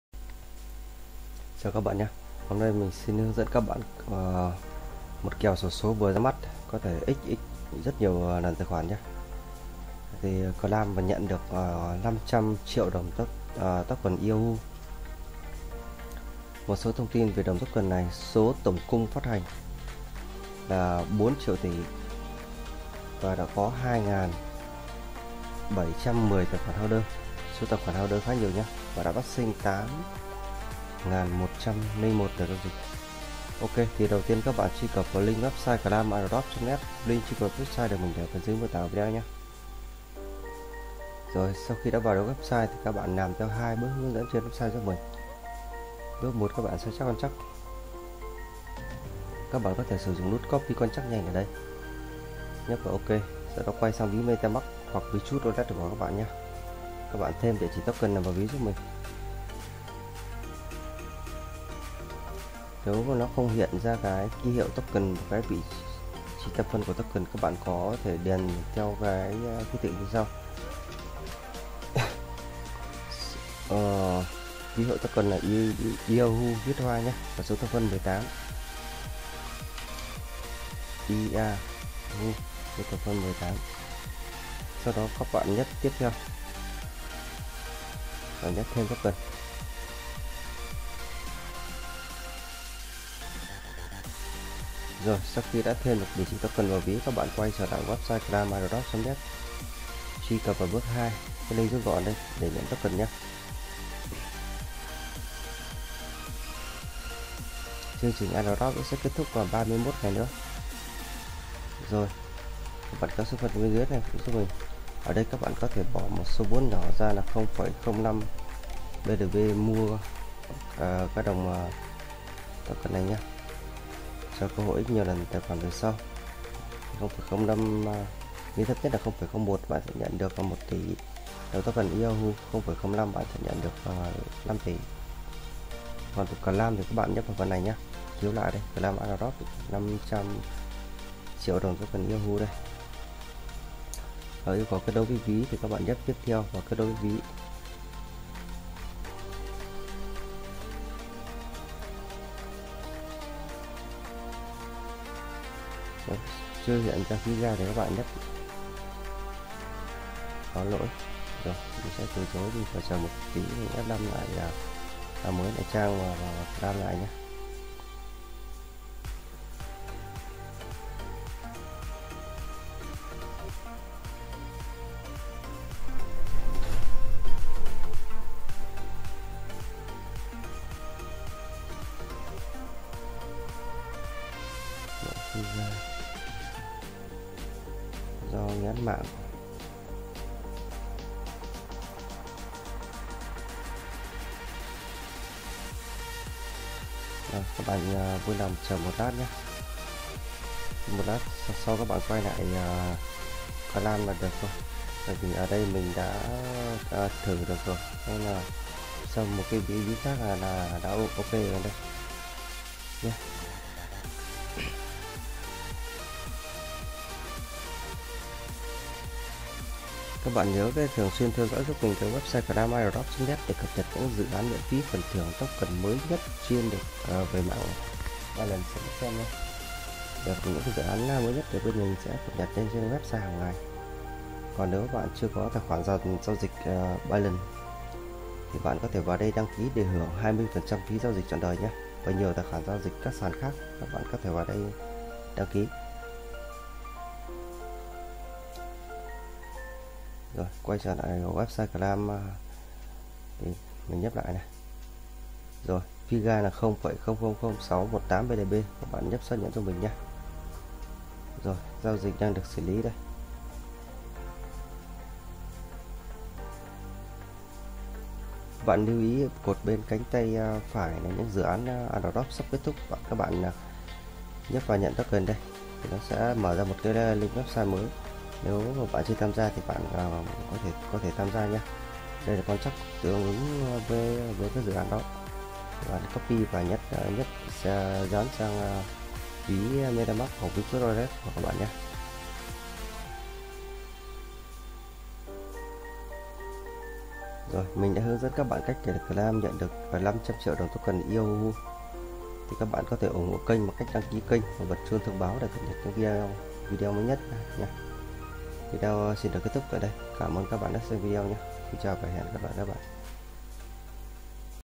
KÈO XỔ SỐ VỪA RA MẮT XXX Tài Khoản, Claim 500 000 000 Yahoo Token, buy 0 01 BNB = 1 Tỷ Yahoo
+ Bước 1: Sao chép Contract :  0xb03e9886c74dcbfb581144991cc6415e46b47e4f

+ Bước 2: Truy Cập Web Tại Đây Click ( Claim Airdrop ) Nhận Token